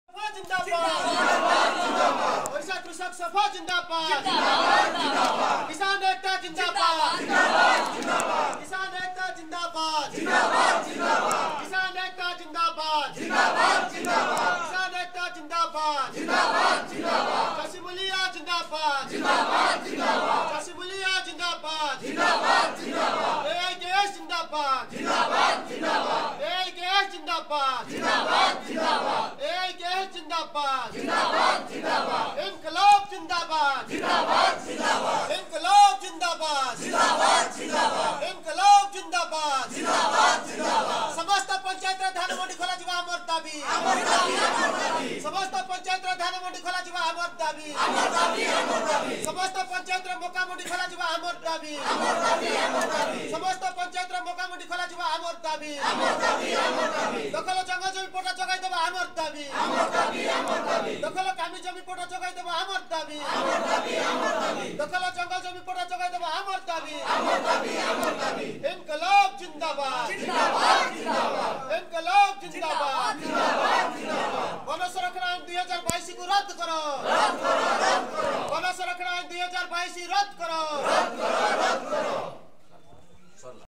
ଓଡ଼ିଶା କୃଷକ ସଭା,ନବରଙ୍ଗପୁର//Odisha Farmers Assembly, Nabarangpur// District Conference Umerkot 2022
ଓଡ଼ିଶା କୃଷକ ସଭା , ନବରଙ୍ଗପୁର//ପ୍ରଥମ ଜିଲ୍ଲା ସମ୍ମିଳନୀ//Odisha Farmers Assembly, Nabarangpur//First District Conference Umerkot 2022

ଉମରୋଟ ହାଟ ପଦାରେ ଆଜି AIKS ଓଡ଼ିଶା କୃଷକ ସଭା ନବରଙ୍ଗପୁର, ଜିଲ୍ଲା ସମ୍ମିଳନୀ ଅନୁଷ୍ଠିତ ହୋଇଛି ଏଥିରେ ରାଜ୍ୟ ସମ୍ପାଦକ ସୁରେଶ ପାଣଗ୍ରାହୀ ଉପସ୍ଥିତ ଥିଲେ।
Communist party of India (Marxist)
otv